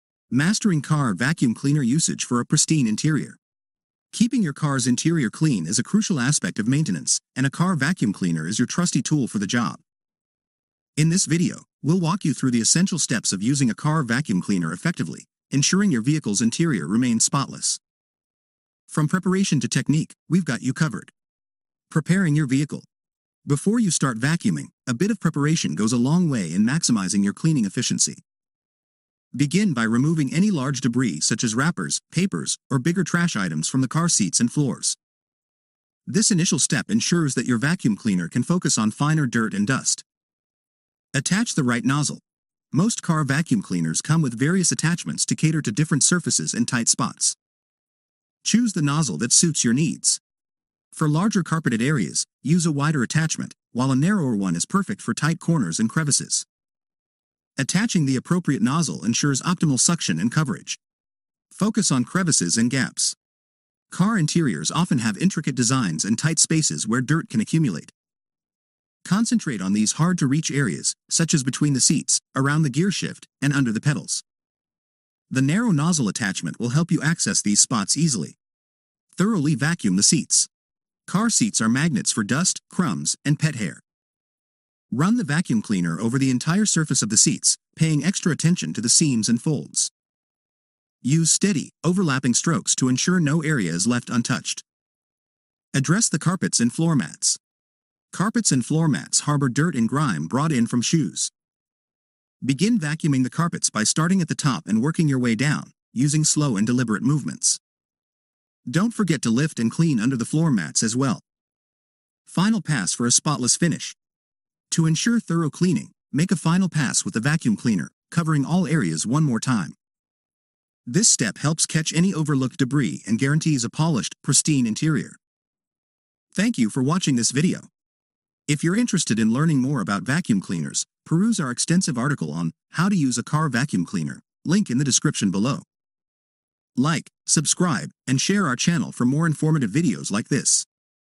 Mastering Car Vacuum Cleaner Usage for a Pristine Interior
In this video, we're going to show you how to achieve a spotless car interior using a vacuum cleaner. Whether you're a car enthusiast or just looking to maintain a clean vehicle, our expert tips and techniques will help you master the art of car vacuum cleaning.

We'll walk you through each step of the process, ensuring that your car's interior remains pristine, free from dust, crumbs, and other debris that can accumulate over time.